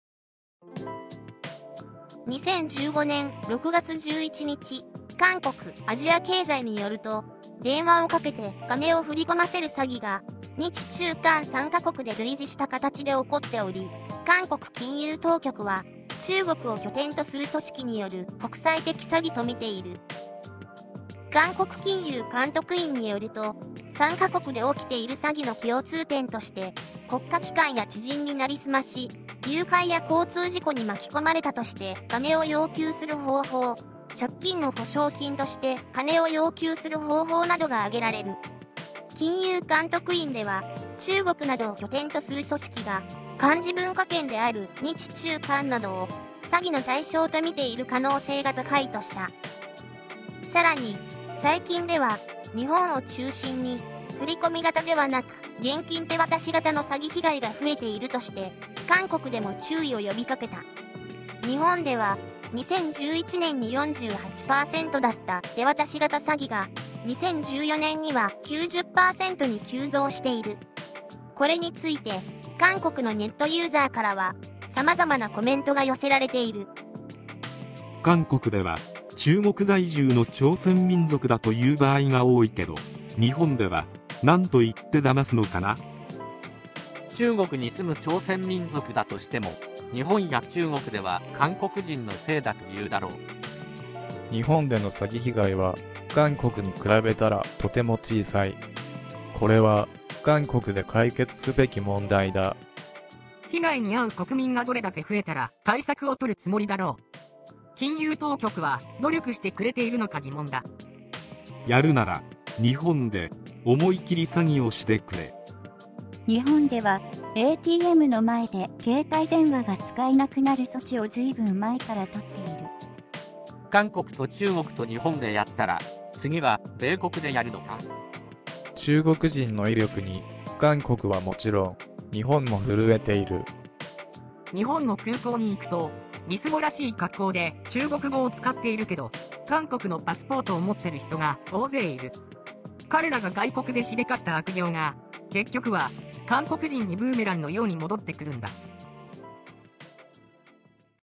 日中韓で振り込め詐欺急増、中国拠点組織が仕業か？―韓国ネットの声
日中韓で類似した形で起こっている振り込め詐欺について、韓国金融当局は、中国を拠点とする組織による国際的詐欺とみている。これについて、韓国のネットユーザーからはさまざまなコメントが寄せられている。